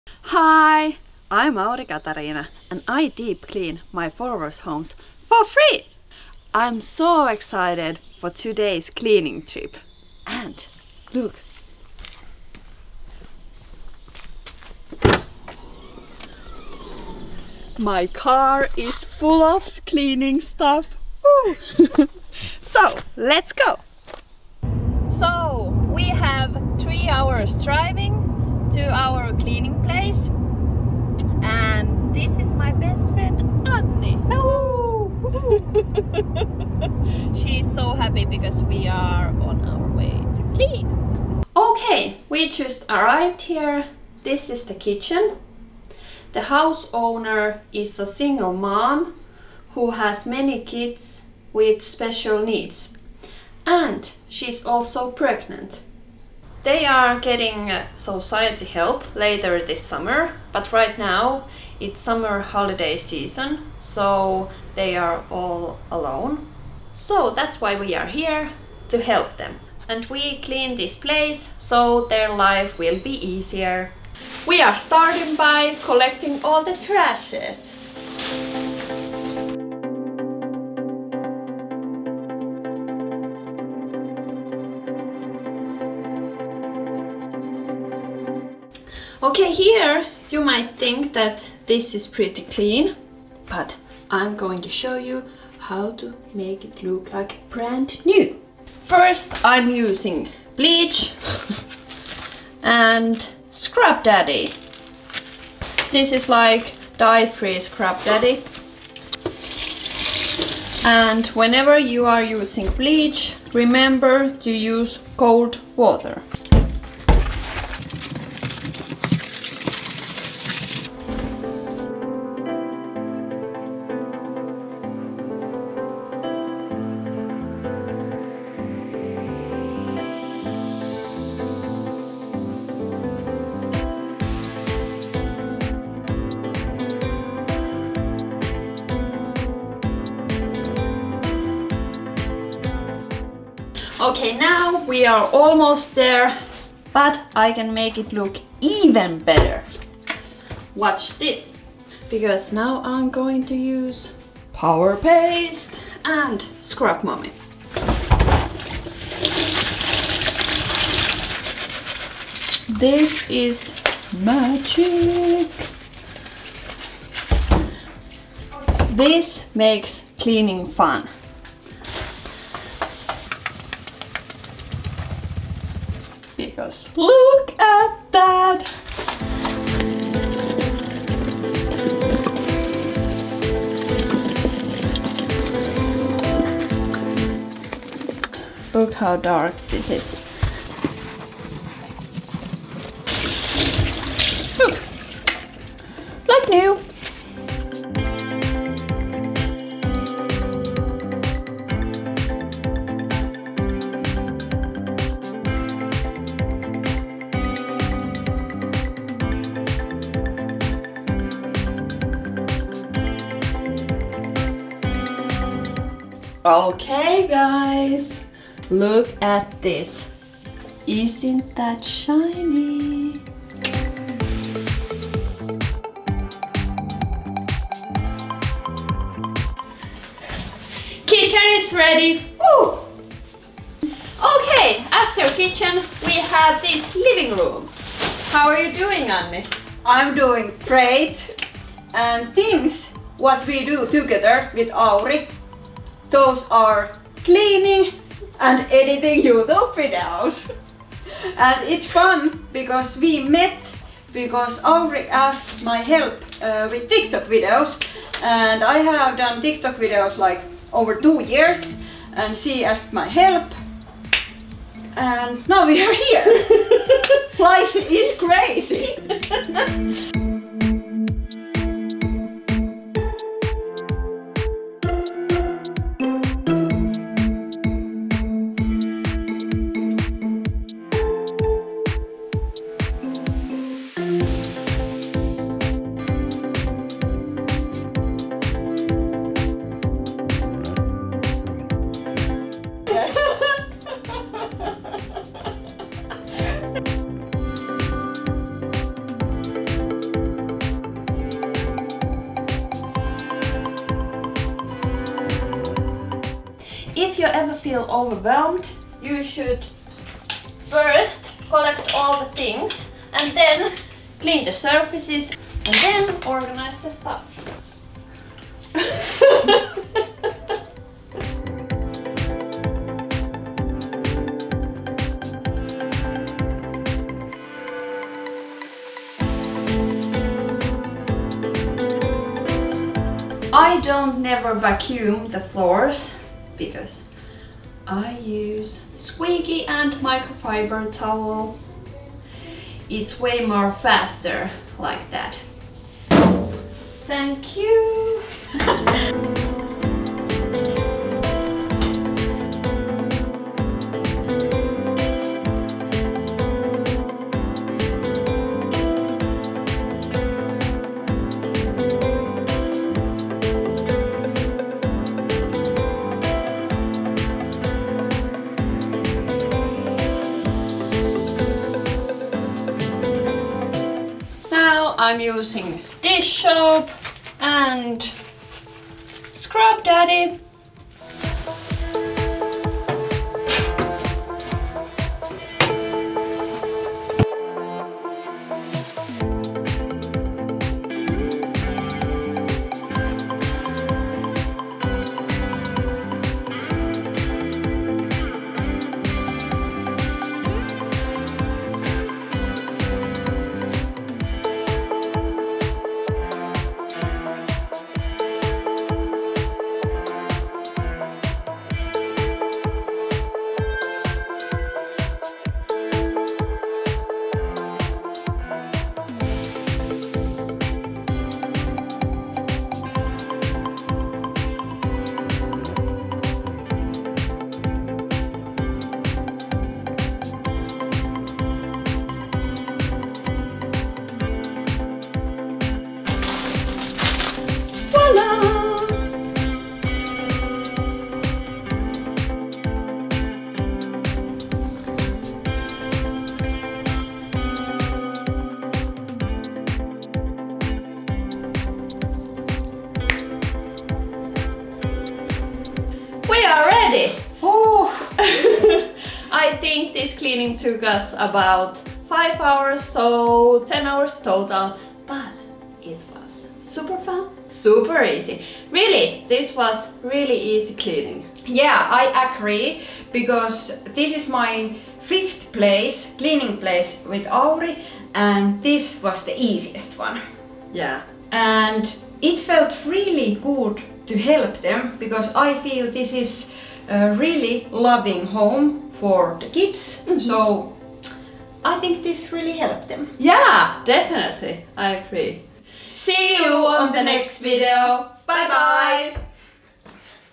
WHOLE HOUSE CLEANING for a single mom and kids 🧸
Hi, I´m Aurikatariina and I do cleaning videos. Today I show you how I cleaned my follower´s home for free! One of my follower asked my help because she is pregnant and needed cleaning help. We cleaned her home for free. Family home makeover with satisfying results! Cleaning tips and cleaning hacks. Kids room cleaning and organizing.

💗Scrub daddy

💗Scrub mommy

💗Alternative "Steel Daddy"

💗Power Paste

💗Oven cleaner

💗Floor Squeegee

💗Microfibre cloths